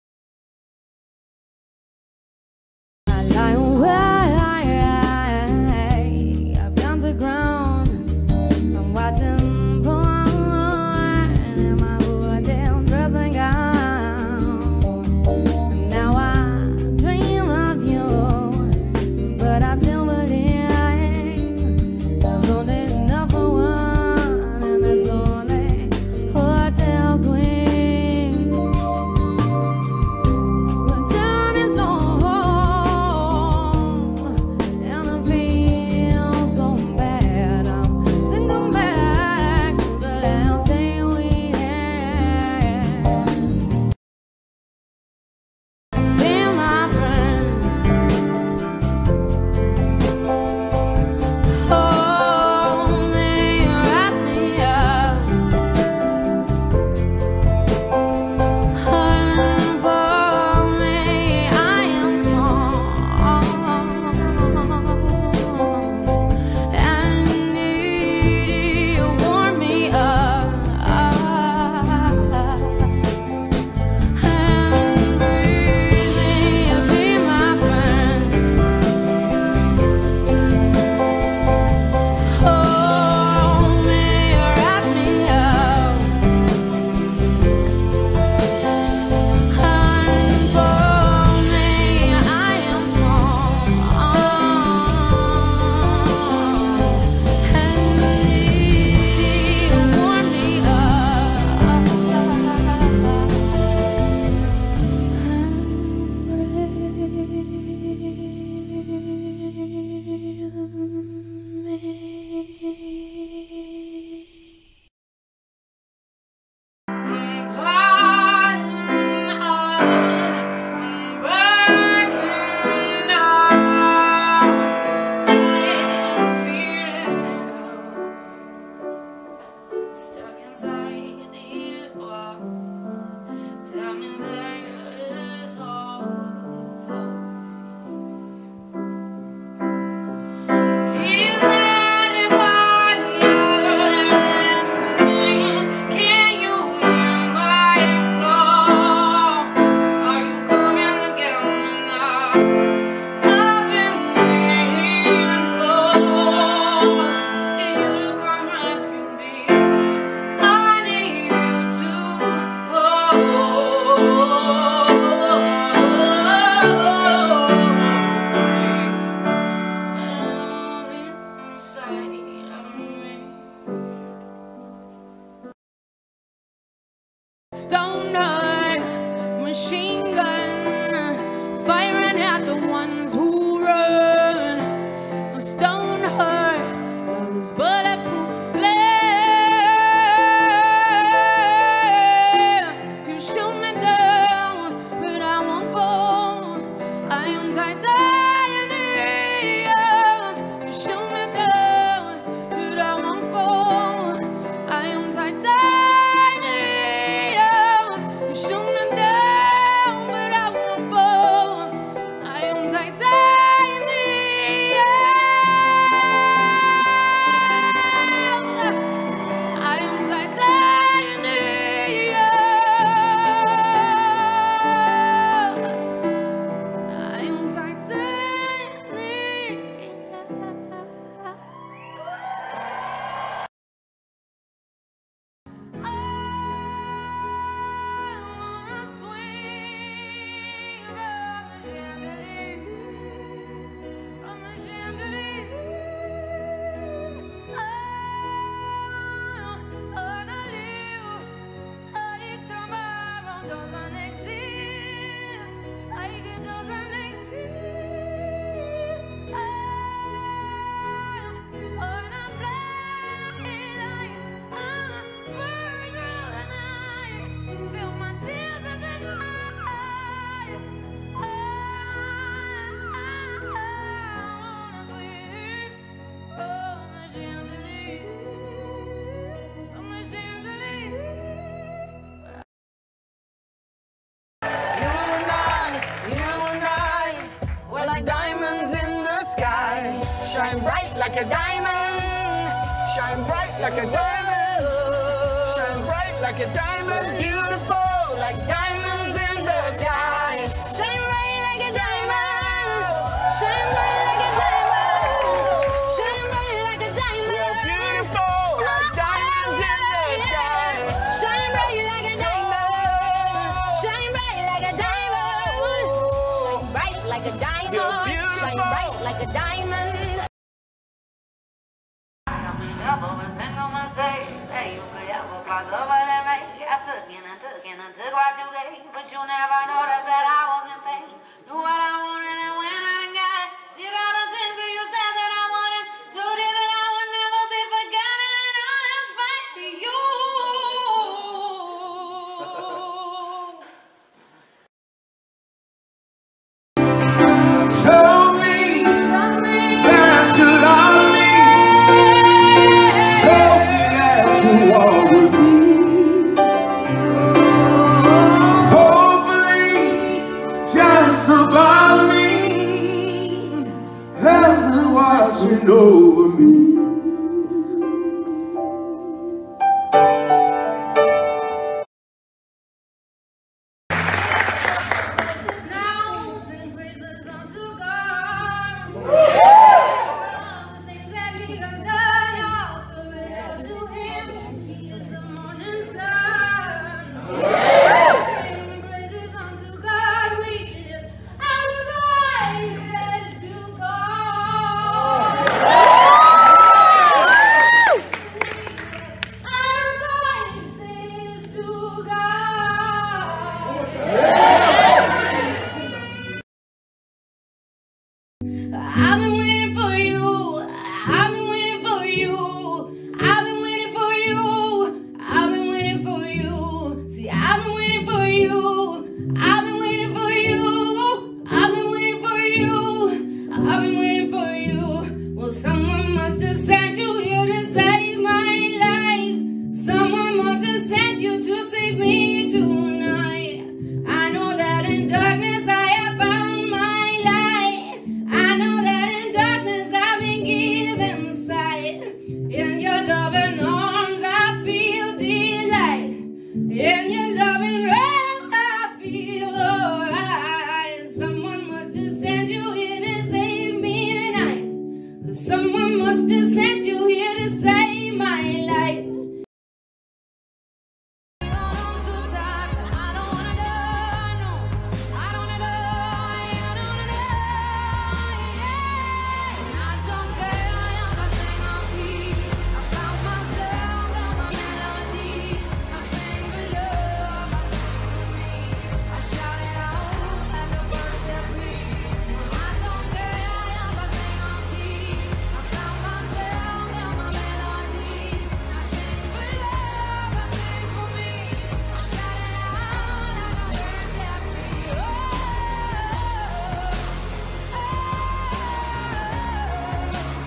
Sia LIVE Evolution! (2002-2020)
Sia is a famous Pop-Singer. But in here career she changed a lot. Her music, style and the voice changed over this 18 years!